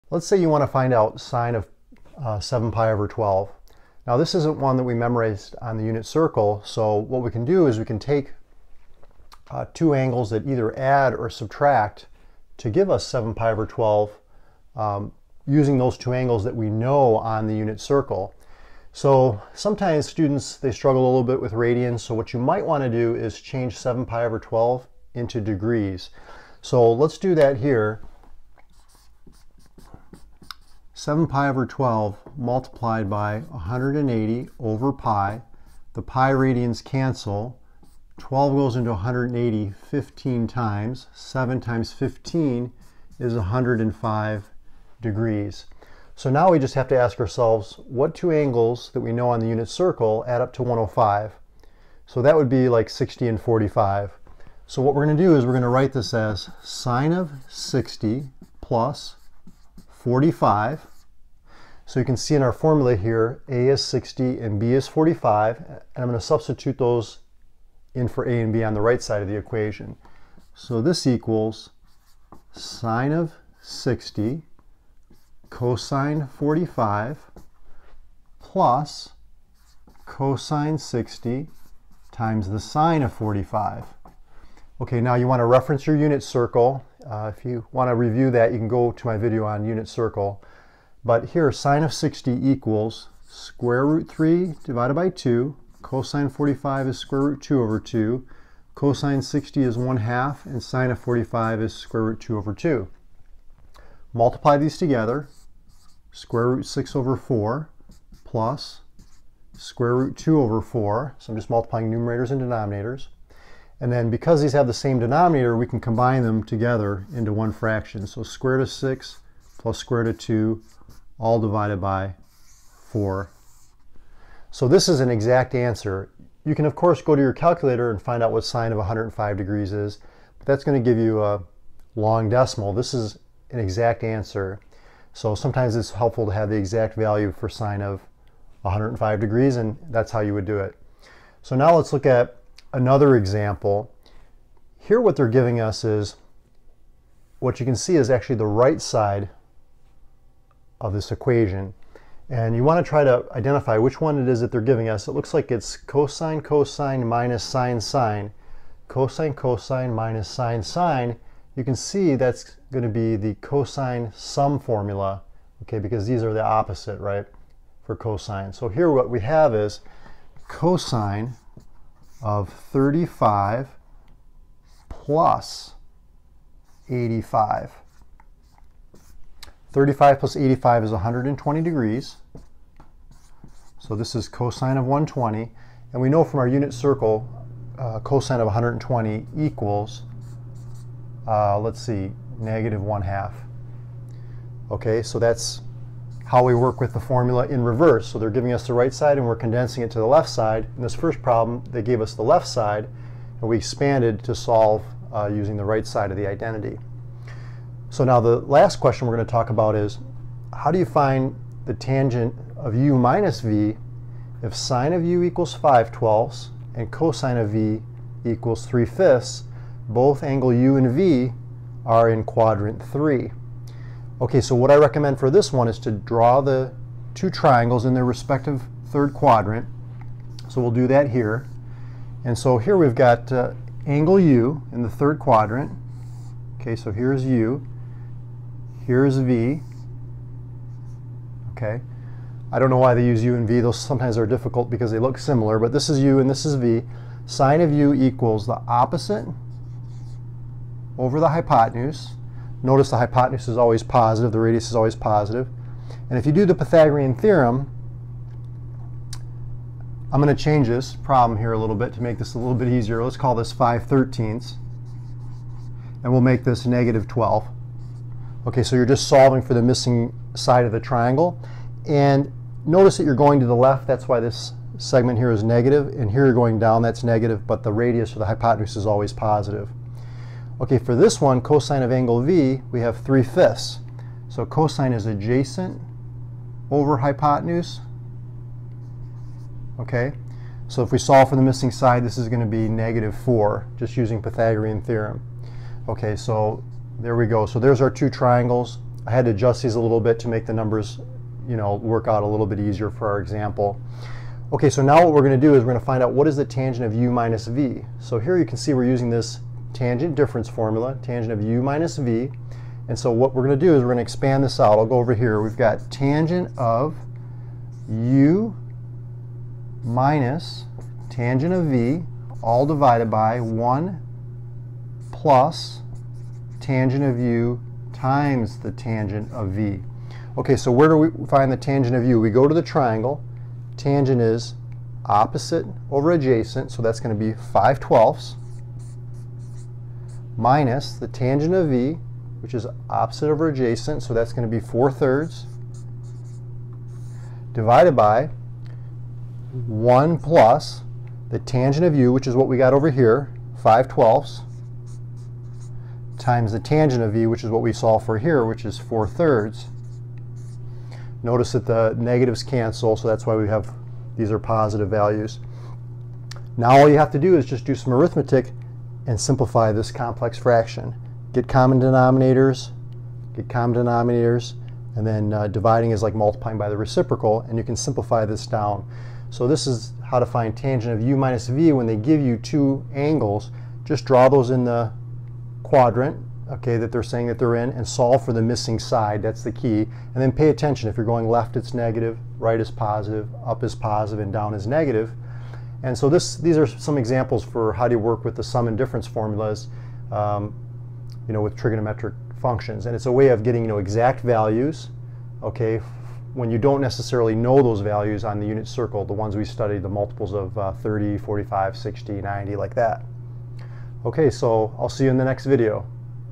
Sum and Difference Formulas
How to use the Sum and Difference Formulas to find the exact value of the sine, cosine or tangent of an angle.

Timestamps:
00:00 Intro
0:12 Formulas for sine, cosine and tangent sum and difference formulas
1:09 Example 1 Finding 7pi/12 using the sine sum formula
3:31 Example 2 looks at condensing a cosine sum expression
4:37 Example 3 looks at working with the tangent difference formula